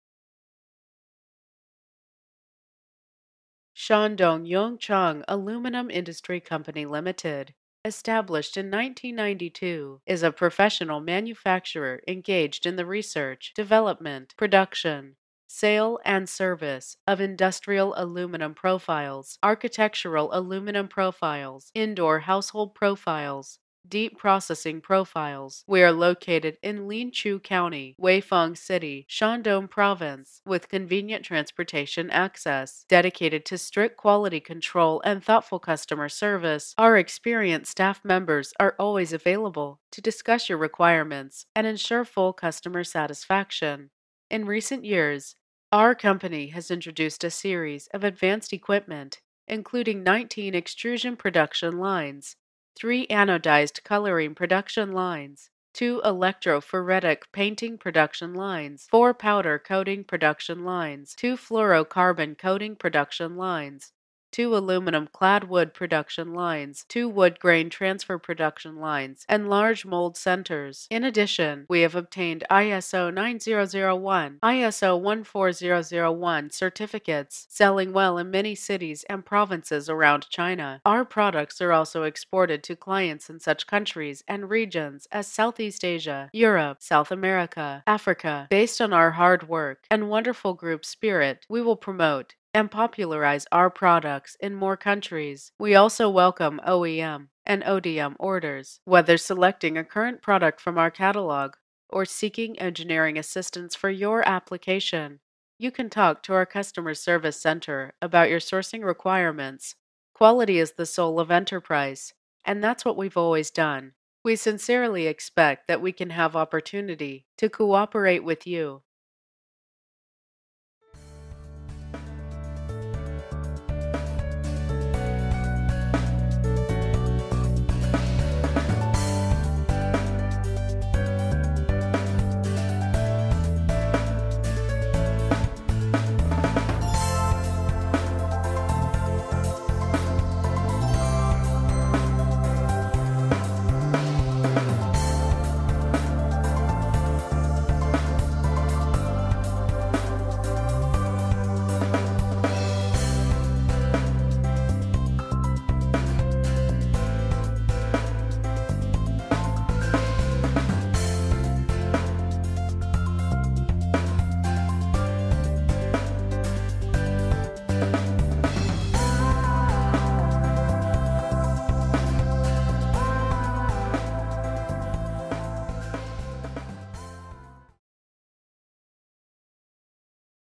Watch The Largest Aluminium Extrusion Company In China!🏭
Shandong Yongchang Aluminium Industry Co., Ltd Video Introduction by Alibaba(شركة شاندونغ Yongchang لصناعة الألمنيوم المحدودة مقدمة الفيديو من قبل علي بابا
About us: Shandong Yongchang Aluminium Industry Co. Ltd. is one of the largest aluminium extrusion company with a legacy spanning 28 years.
The company's core business is in aluminium extrusions, such as window and door's frame, curtain wall, solar frame and so on.

Our Products: Aluminium extrusion profile for curtain wall, windows, doors, industry, kitchen.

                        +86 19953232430